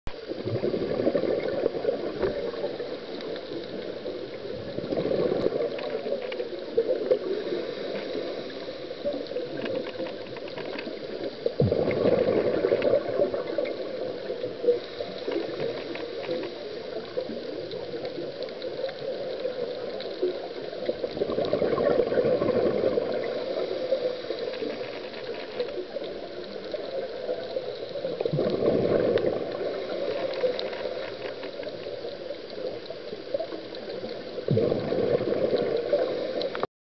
The spadefish whisperer
My friend Karen working her magic on a pair of longfin spadefish in Fiji.